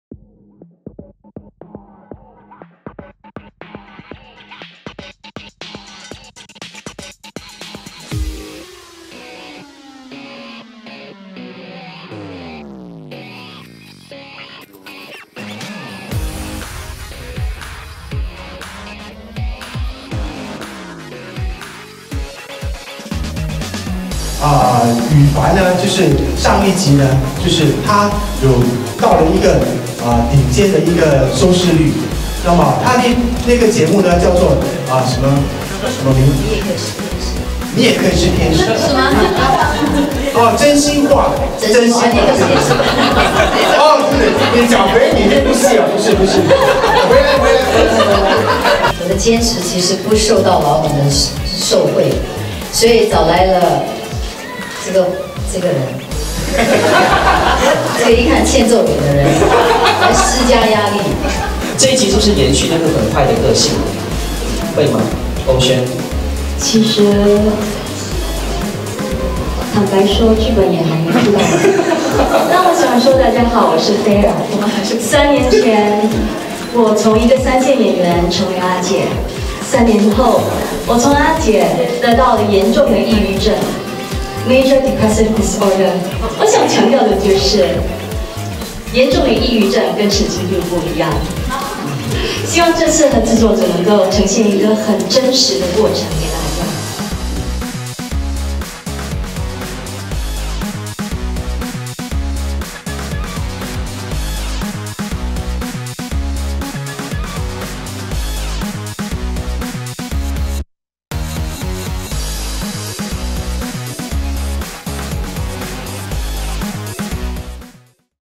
Zoe Tay, Li Nan Xing and Jeanette Aw tell all at the Dream Makers 2 Lensing!
Dream Makers is back by popular demand! Zoe seems to be living the dream... maybe someone else's? And don't get Jeanette Aw angry!

www1.toggle.sg/thecelebrityagency